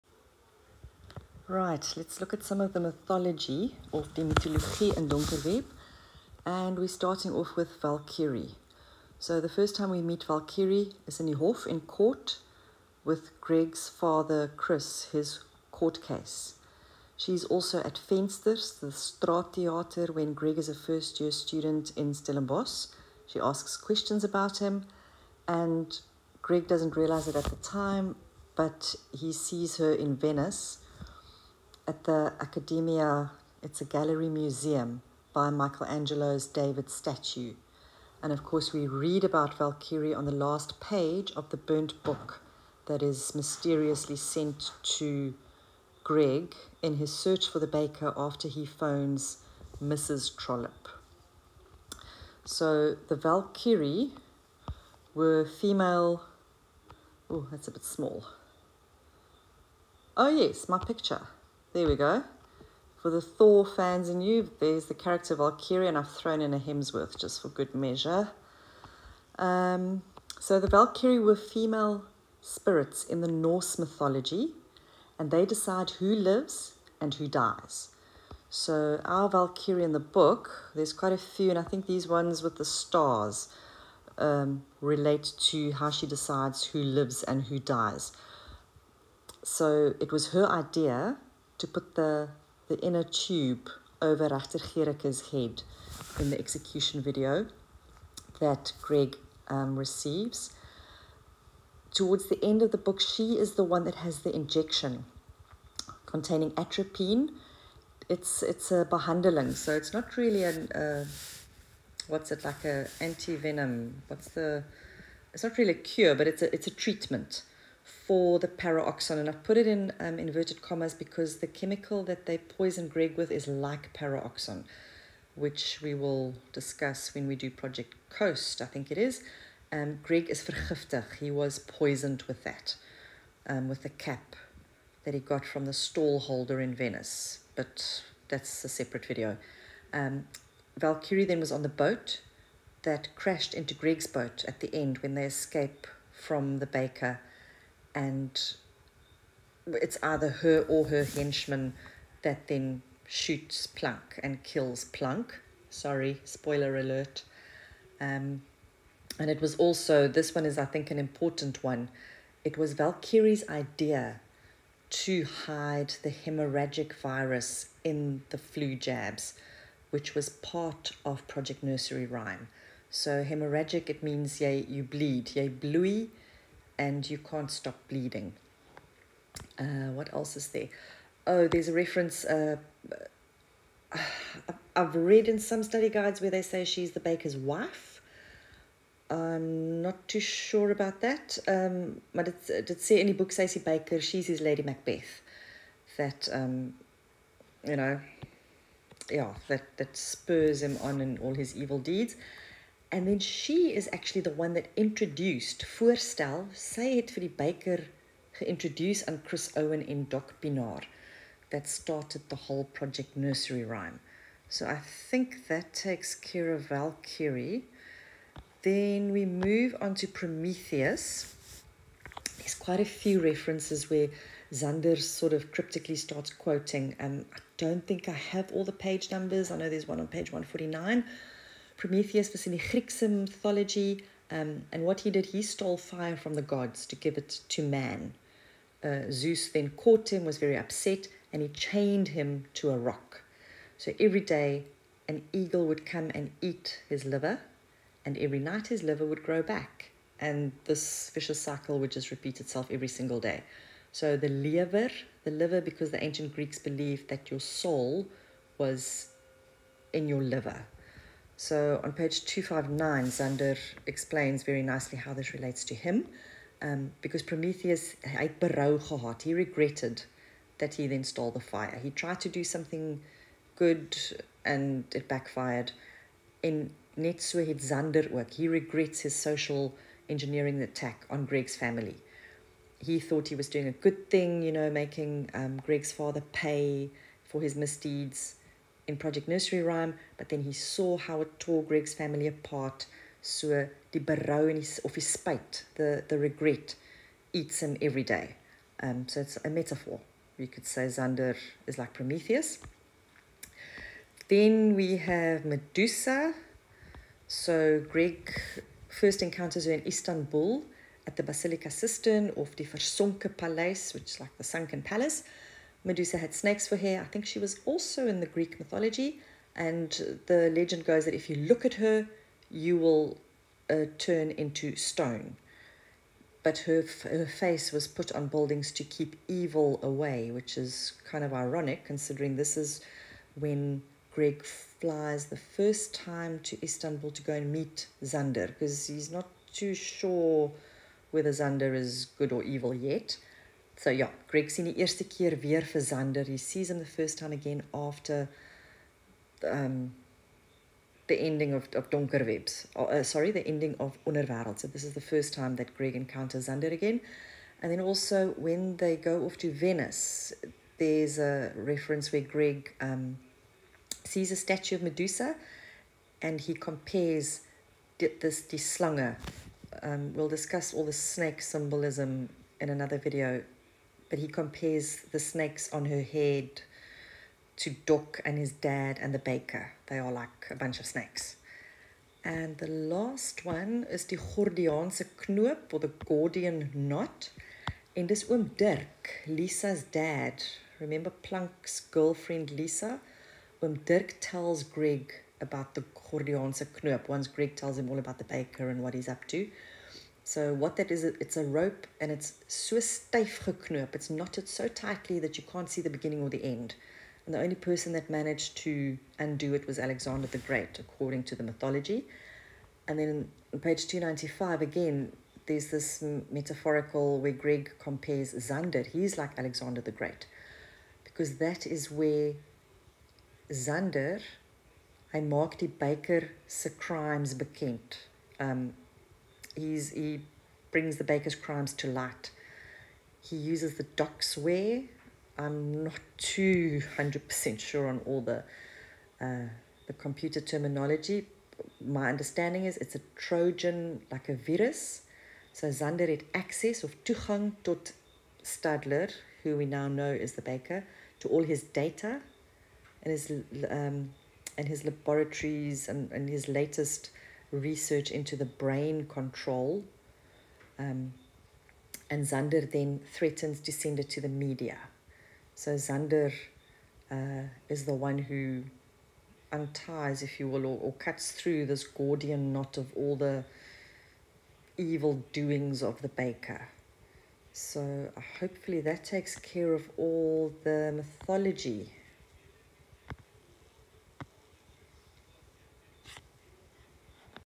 Donker Web Mitologie
!!! CONTAINS SPOILERS!!!
An explanation of the mythology: Valkyrie, Prometheus, Medusa and the Gordian Knot